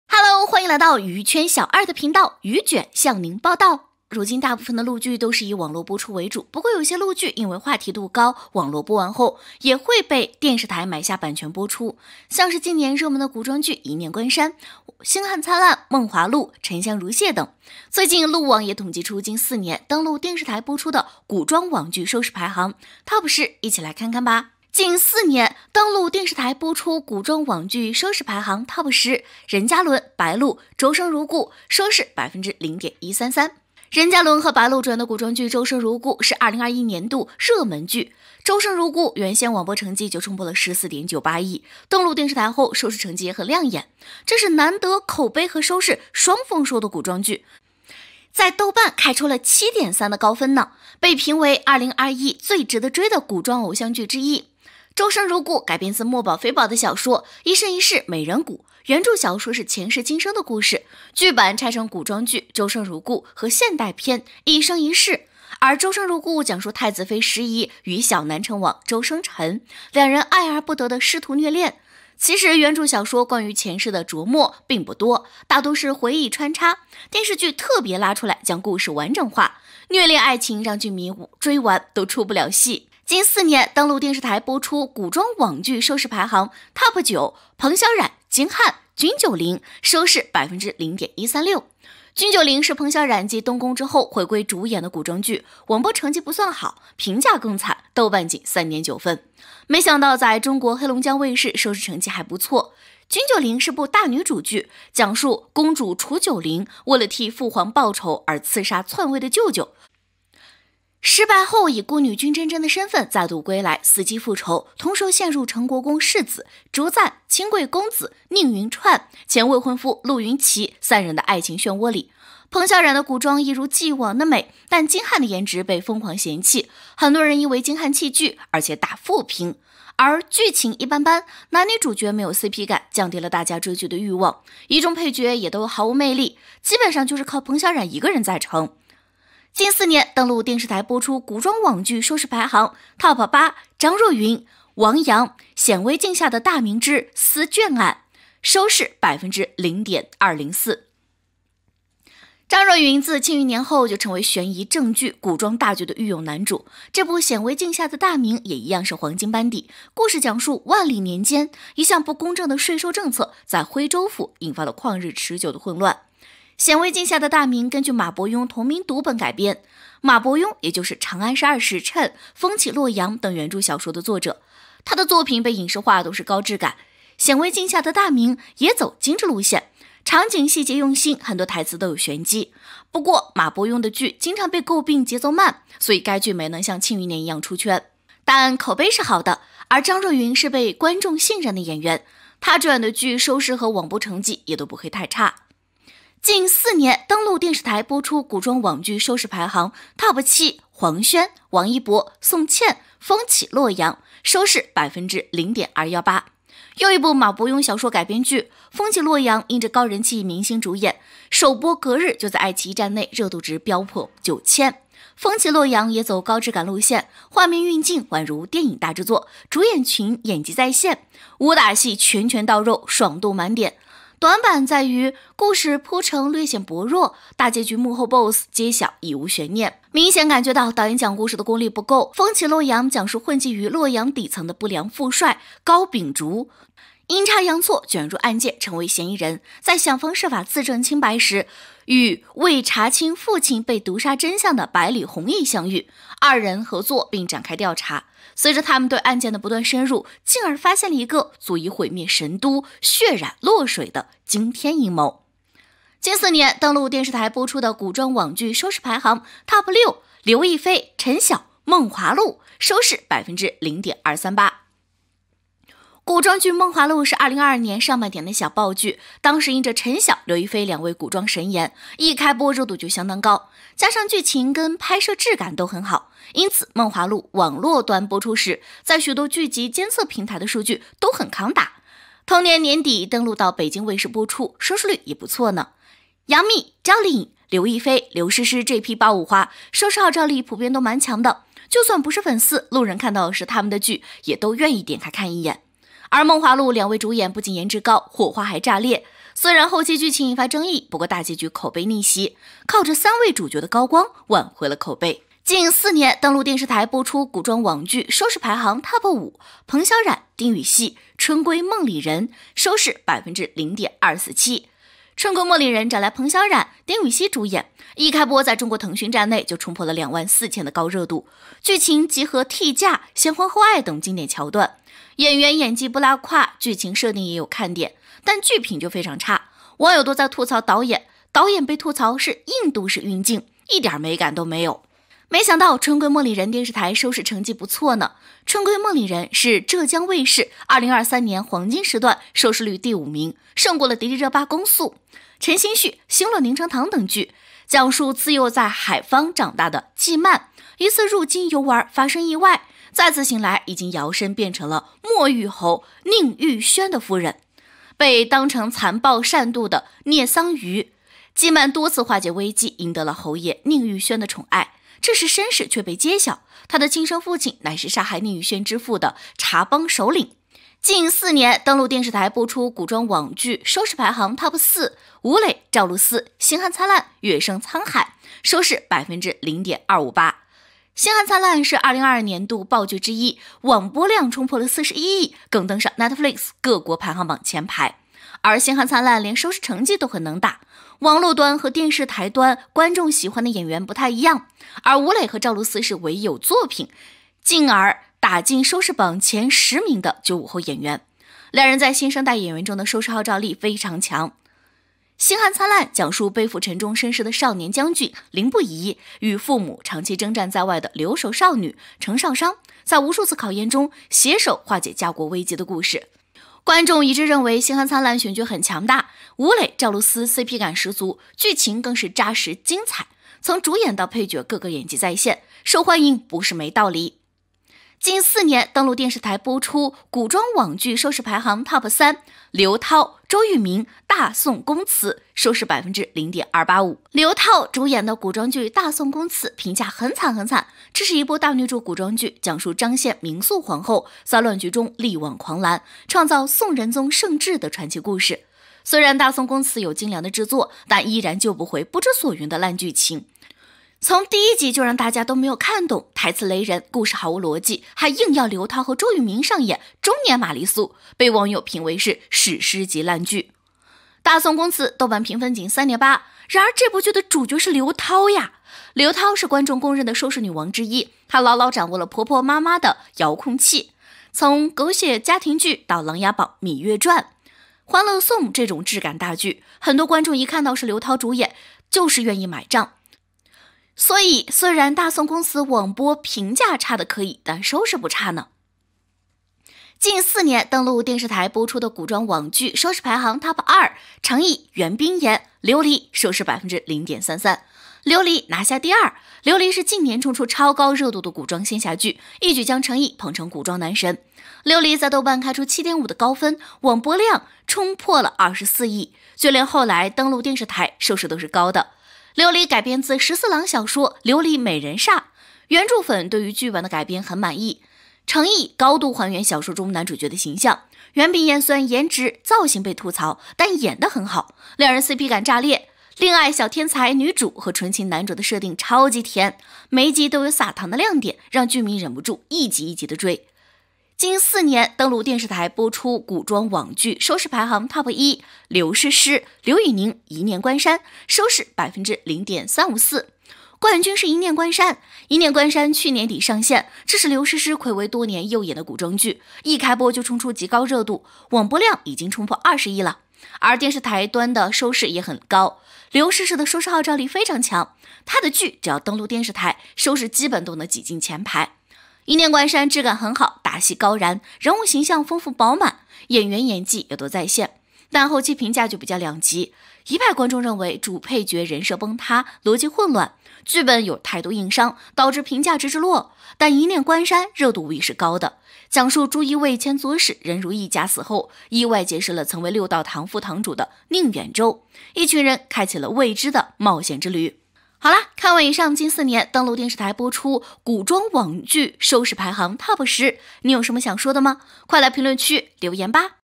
一登電視臺就現原形！白鹿任嘉倫墊底，成毅袁冰妍第2？近四年古裝網劇「上星」收視TOP10！黃軒王一博倒數，吳磊趙露思第4？第一名她的劇基本都能進前排！任嘉倫
如今大部分的陸劇都是以網路播出為主，
不過有些陸劇因為話題度高，網路播完後，
也會被電視臺買下版權播出。
像是近年熱門的古裝劇《一念關山》、
《星漢燦爛》、《夢華錄》、《沉香如屑》等。
一登電視臺就現原形！
白鹿任嘉倫墊底，成毅袁冰妍第2？
近四年古裝網劇「上星」收視TOP10！
黃軒王一博倒數，吳磊趙露思第4？
第一名她的劇基本都能進前排！